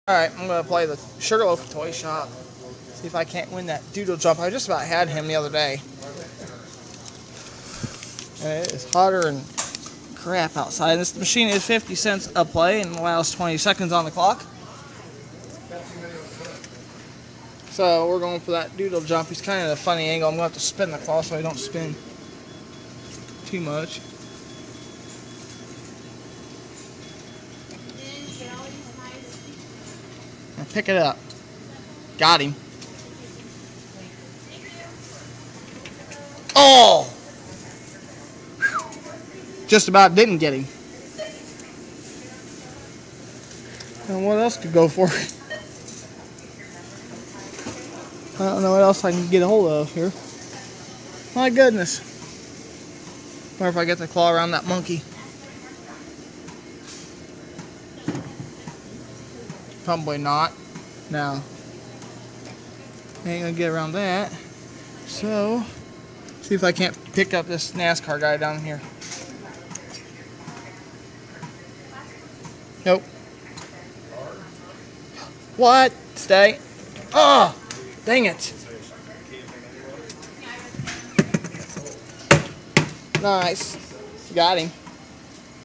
( SUGAR LOAF) TOY SHOPPE CLAW MACHINE @KMART ALMOST DIDNT GET IT
THIS WAS FILMED ALMOST 2 WEEKS AGO AT Y LOCAL KMART HERE I AM PLAYING THE TOY SHOPPE HOPE YOU ENJOY